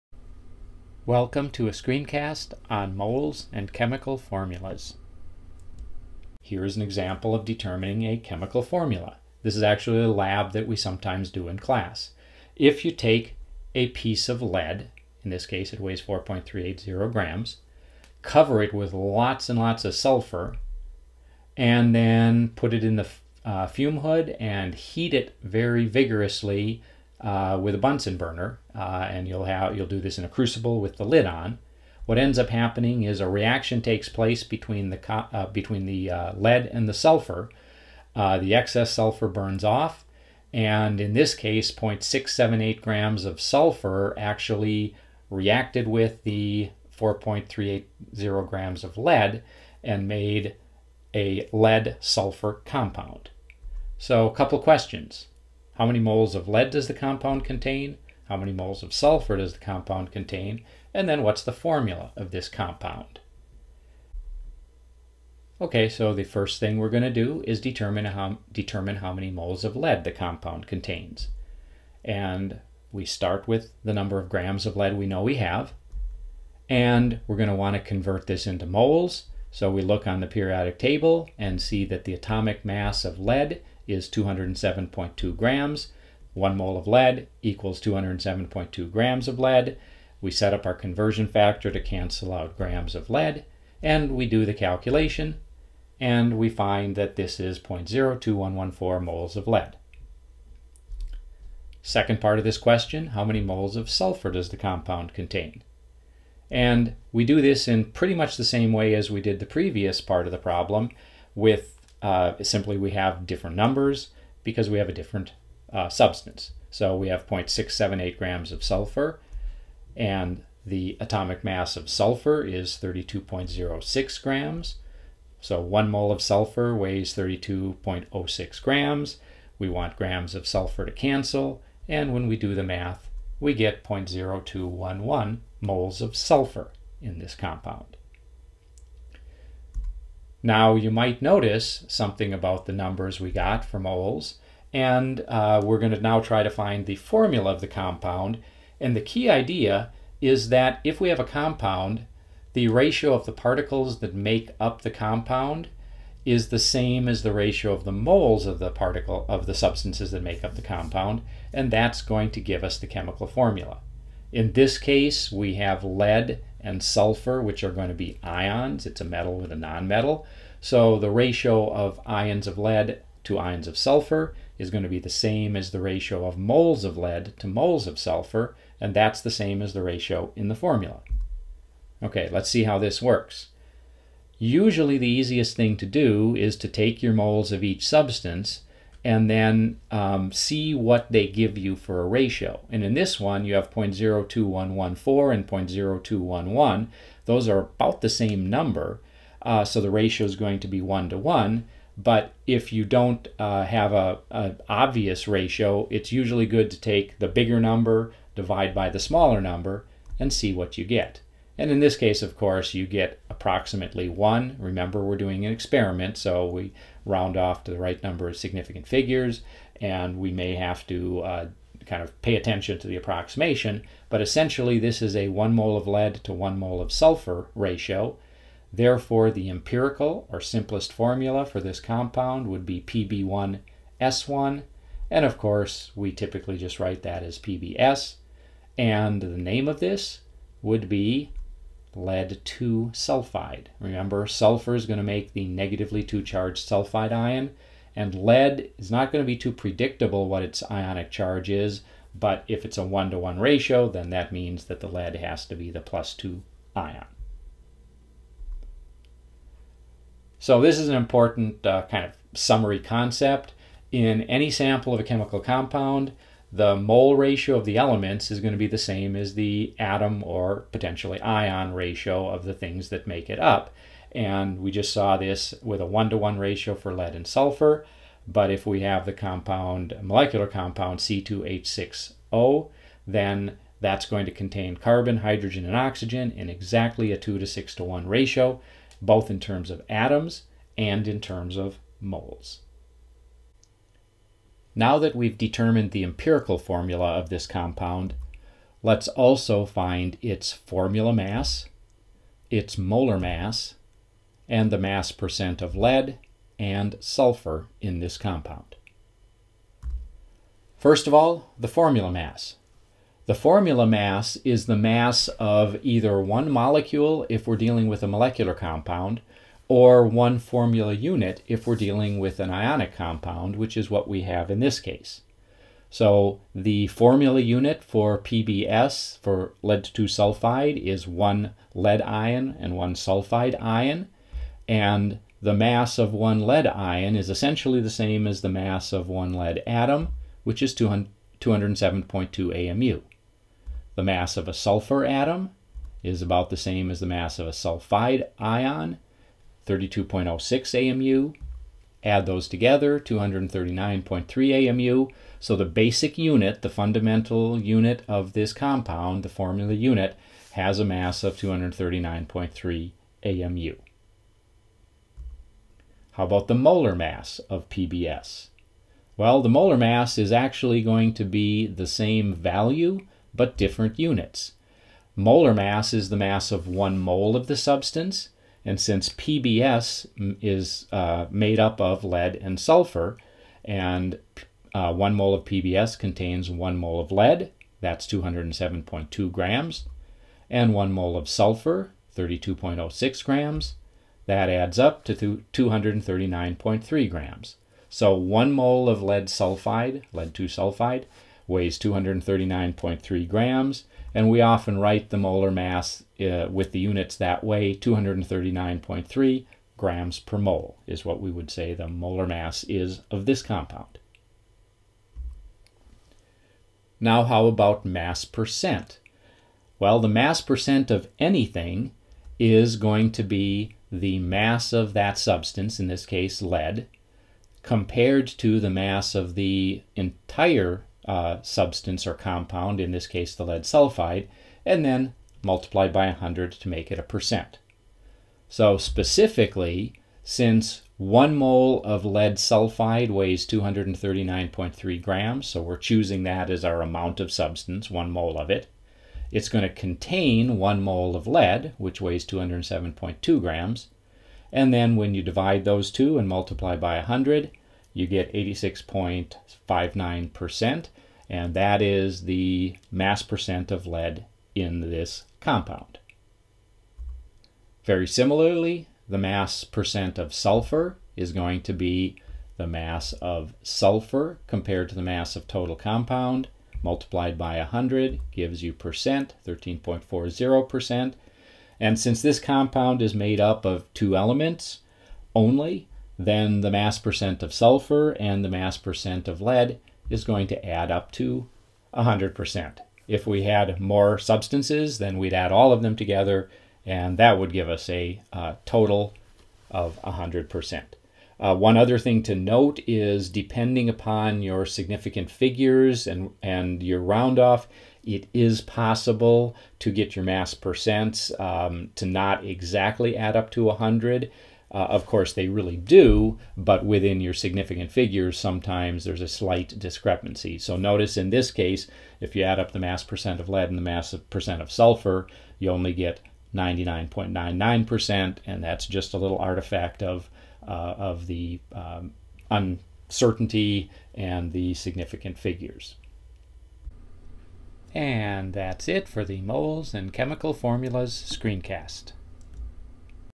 Moles-and-Chemical-Formulas-Screencast-F16
Introduction to using mole ratios to determine chemical formulas, along with percent composition calculations